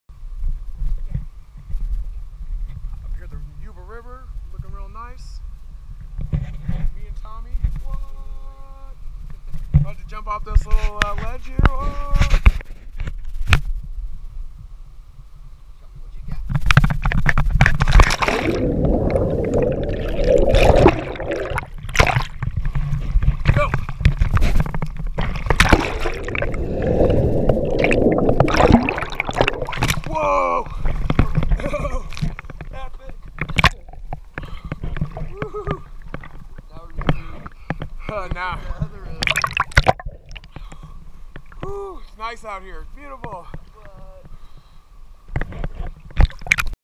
The Yuba River California , Liam O'Connor and friends
Rock jumping on the river